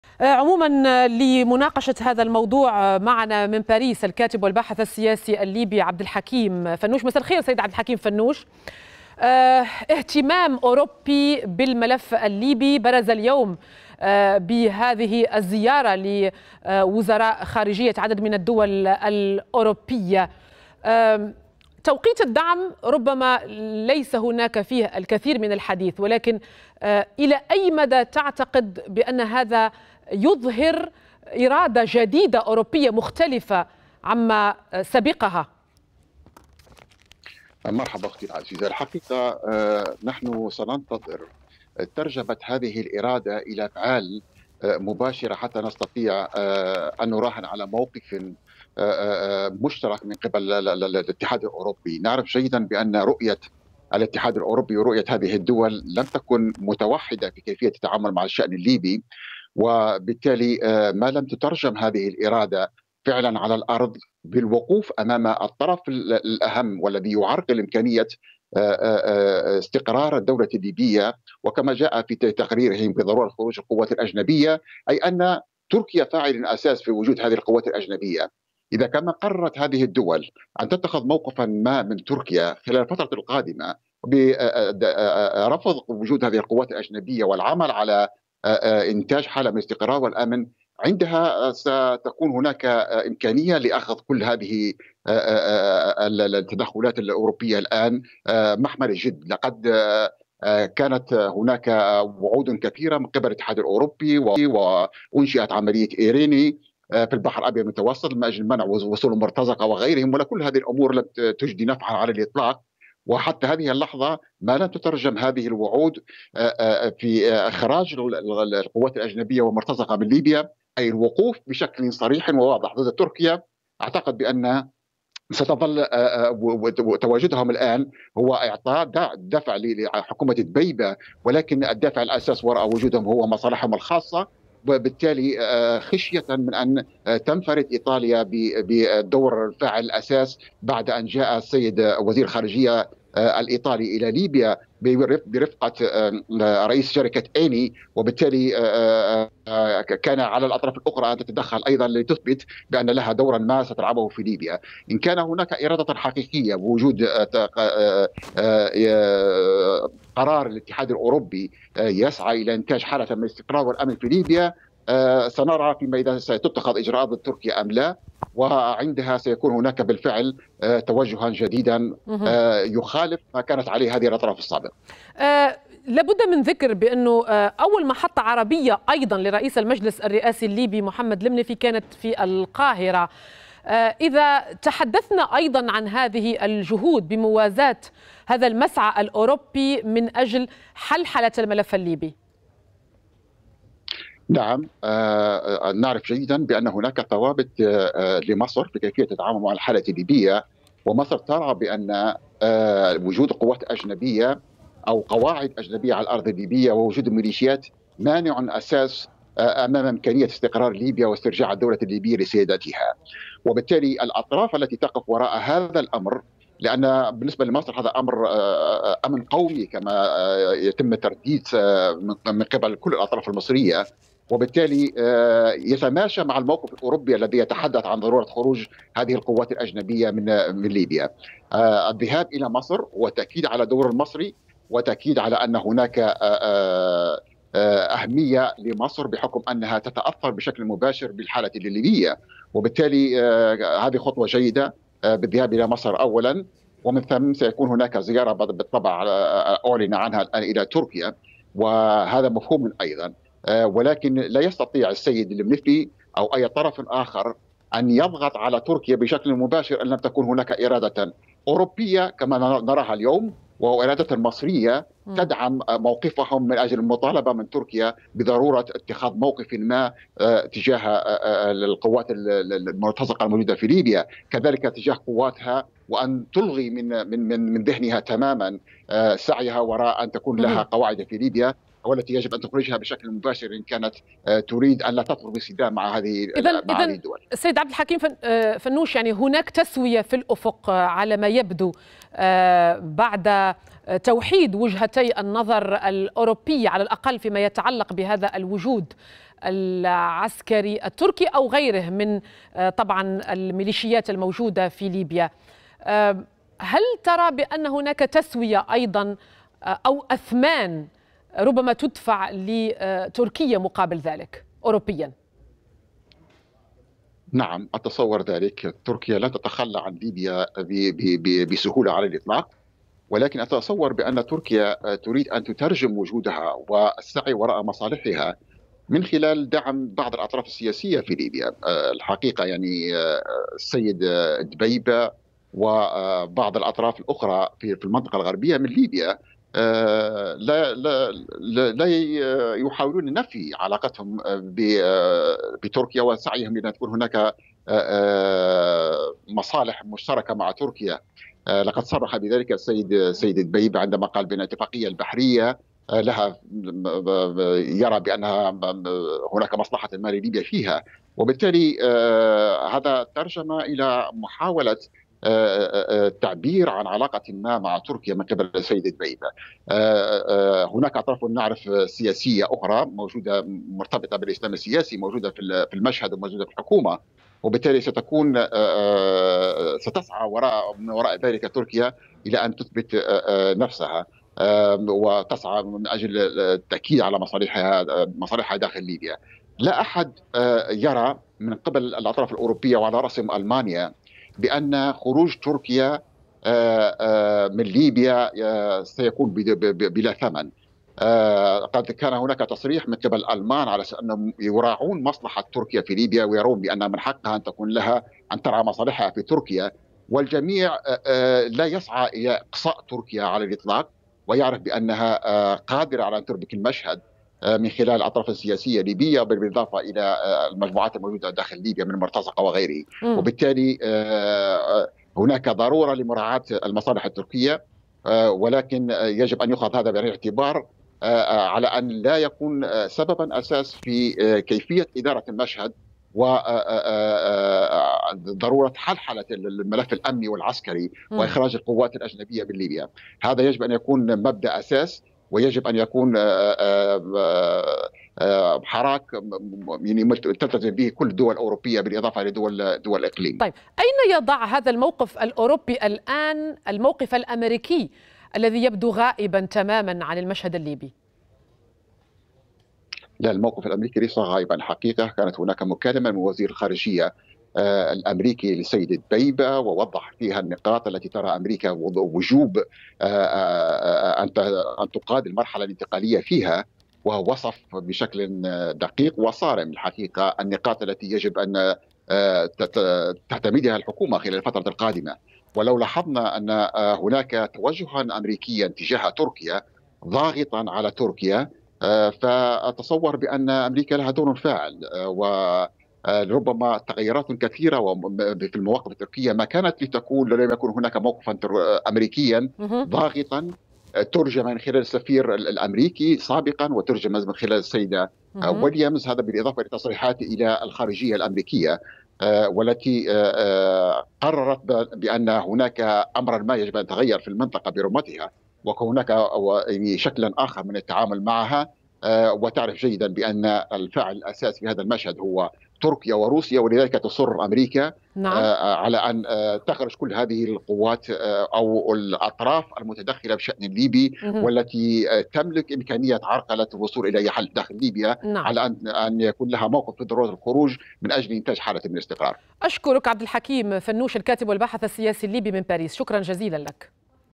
عبد الحكيم فنوش: رؤية الدول الأوروبية تجاه الشأن الليبي لم تكن موحده
الكاتب والباحث السياسي الليبي عبد الحكيم فنوش أشار الى إن رؤية الدول الأوروبية كانت مختلفة أتجاه الملف الليبي الأن يجب ان تترجم تلك الرؤية لإفعال حقيقية لمواجهة المعرقل الأساسي للعملية السياسية في ليبيا الا وهي الميليشيات يزور وزراء خارجية فرنسا وألمانيا وإيطاليا طرابلس الخميس 03/25 حيث سيعقدون مؤتمرا صحافيا مشتركاً مع نظيرتهم الليبية في العاصمة الليبية طرابلس، في إطار الدعم الأوروبي للسلطة التنفيذية الجديدة التي تم اختيارها في جنيف الشهر الماضي. وتأتي هذه الزيارة المشتركة بعد أقل من أسبوعين على تشكيل حكومة ليبية موحدة جديدة مسؤولة عن إدارة المرحلة الانتقالية، وصولا إلى الانتخابات العامة المقررة في نهاية العام الحالي